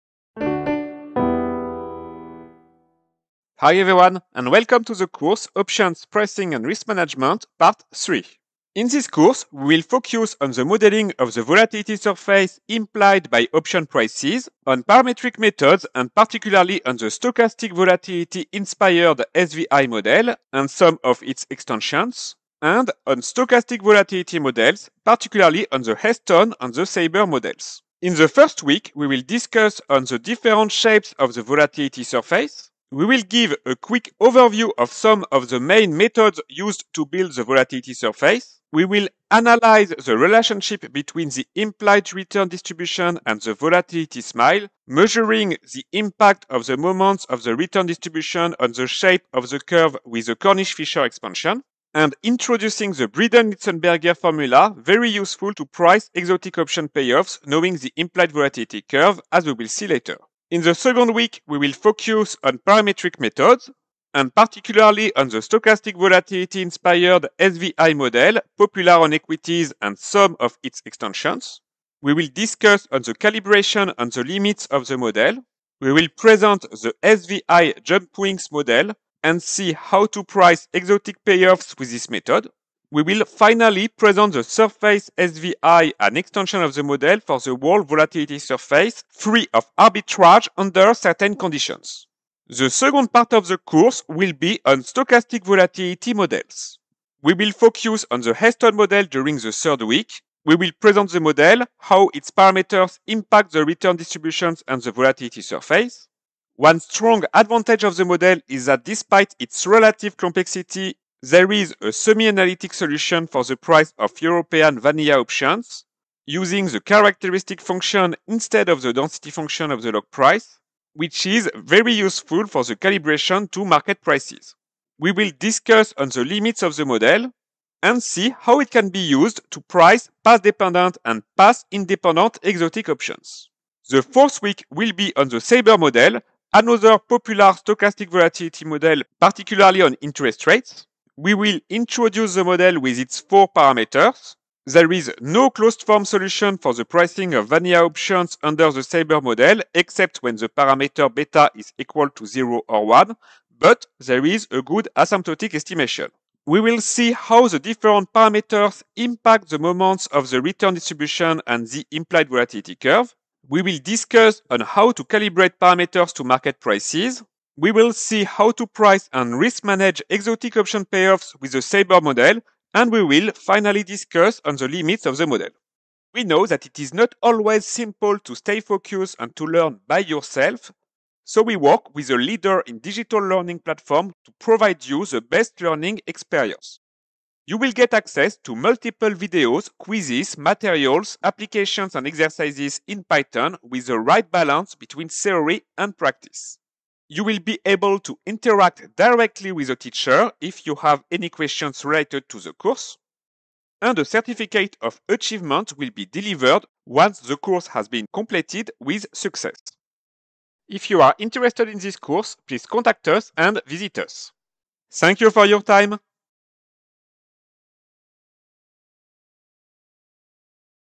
Options, Pricing and Risk Management Part III - Course Overview
This third part of our course “Options, Pricing and Risk Management” will be on the modelling of the volatility surface,  parametric models with a focus on the SVI model, and stochastic volatility models with a focus on the Heston and the SABR models.

The course is composed of many videos, quizzes, applications and tutorials in Python.

A certificate of achievements will be delivered once the course has been completed with success.

In the first week, we will discuss the limits of the Black-Scholes model, particularly the fact that the volatility surface is not flat in practice. We will give an introduction on the modelling of the volatility surface implied by option prices with a quick overview of some of the main methods used to build the volatility surface. We will present the Newton-Raphson method for extracting the implied volatility from option prices.

We will measure the impact of the skewness and kurtosis of the return distribution on the volatility smile using the Cornish Fisher expansion, and we will introduce the Breeden-Litzenberger formula which links the second order derivative of vanilla option prices with the risk-neutral density function and can be very useful to price exotic option payoffs.

In the second week, we will focus on parametric methods and particularly on the Stochastic Volatility Inspired (SVI) model, popular on equities and some of its extensions. We will discuss on the calibration and the limits of the model.

The SVI Jump-Wings (SVI-JW) parameterisation is an interesting alternative method where the different parameters have a concrete interpretation, each of them controlling a specific aspect of the smile, making model calibration easier and more robust. We will see how to price exotic payoffs with this method. We will finally present the Surface SVI (SSVI) an extension of the model for the whole volatility surface, free of arbitrage under certain conditions.

The second part of the course will be on stochastic volatility models.

We will focus on the Heston model during the third week,  which is one of the most used stochastic volatility model. The model assumes that the variance of the asset price is stochastic, it is correlated with the asset price and follows a mean-reverting process. We will present the model, how its parameters impact the return distributions and the volatility surface. One strong advantage of the model is that despite its relative complexity, there is a semi-analytic solution for the price of European vanilla options using the characteristic function instead of the density function of the log price, which is very useful for the calibration to market prices. We will discuss on the limits of the model and see how it can be used to price path-dependent and path-independent exotic options.

The fourth week will be on the SABR model, another popular stochastic volatility model, particularly on interest rates. The model assumes that the forward price and its variance are both stochastic and correlated. We will introduce the model with its four parameters. There is no closed-form solution for the pricing of vanilla options under the SABR model except when the parameter beta is equal to zero or one but there is a good asymptotic estimation. We will see how the different parameters impact the moments of the return distribution and the implied volatility curve. We will discuss on how to calibrate parameters to market prices, on the limits of the asymptotic estimate and we will see how to price and risk manage exotic option payoff with the SABR model. We will finally discuss the limits of the model.